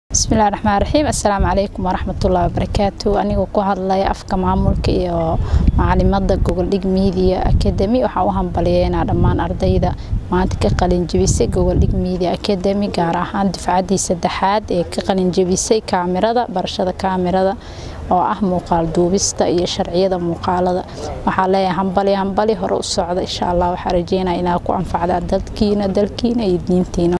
Mamulka ahna macalimada Gogoldhig ustaad Iqra Abdulle Gure oo Hambalyo udirtay ardayda
Mamulka ahna macalimada Gogoldhig ustaad Iqra Abdulle Gure oo Hambalyo udirtay ardayda ka qalin jabisay barashada camerada
Si aad oga mid noqoto ardayda cusub
Gogoldhig Academy "Baro xirfad oo Bari Shaqo Tag"